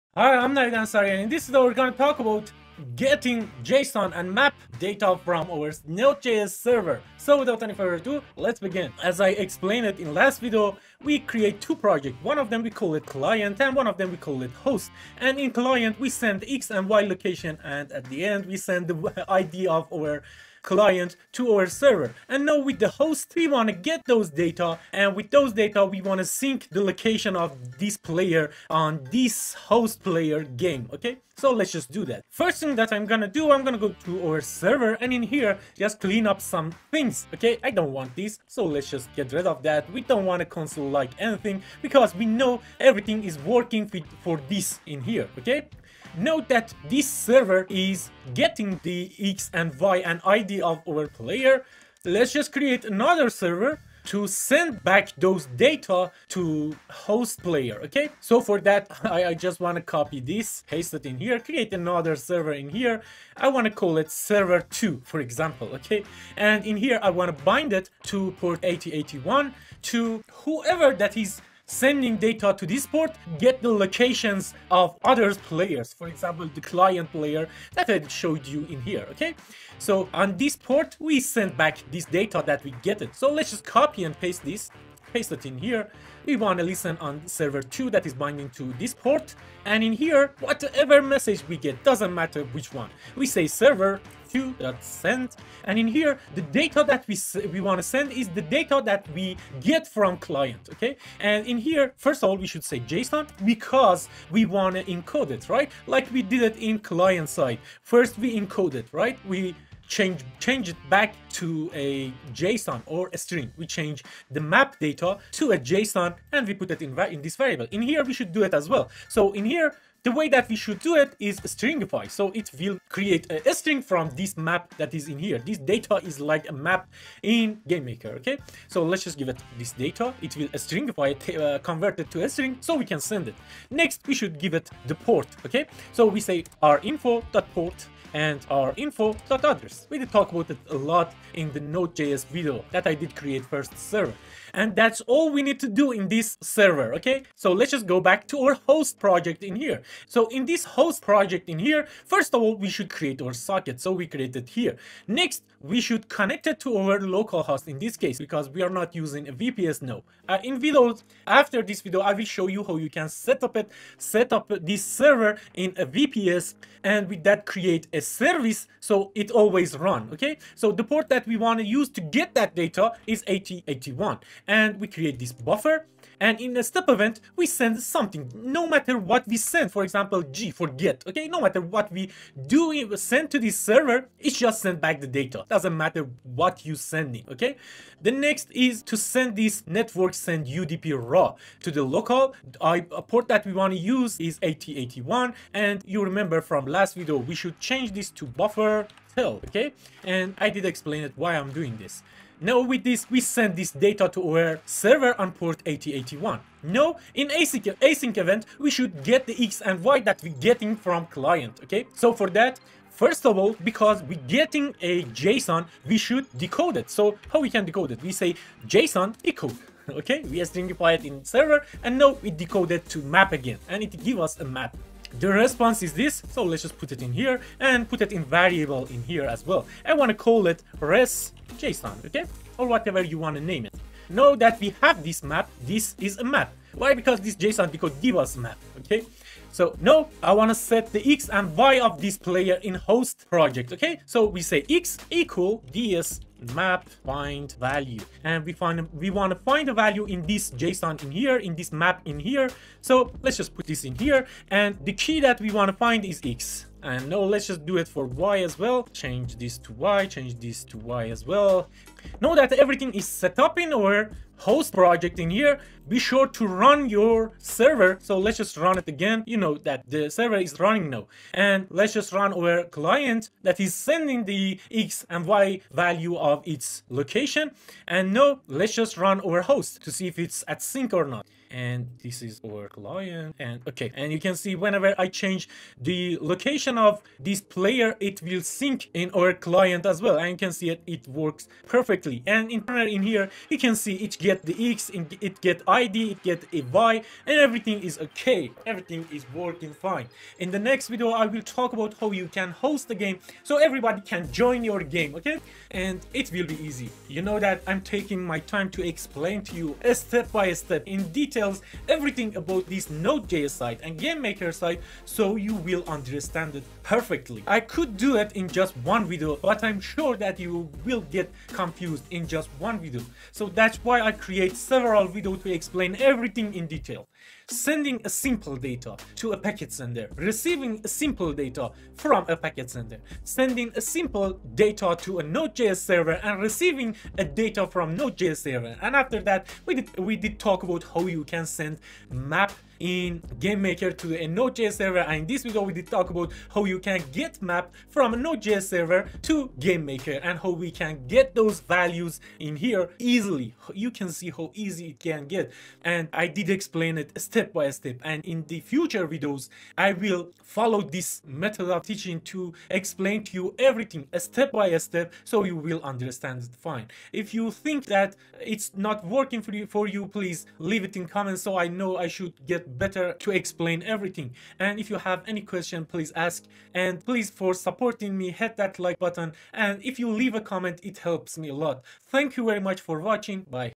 Multiplayer game use json and map to sync location of players
get json data from server and convert json to map with async network event in gamemaker studio

timecodes:
0:00 intro
0:50 create a server in node js to send data to gamemaker
1:41 convert map to json in nodejs
3:27 use json_decode to convert json to map in gamemaker studio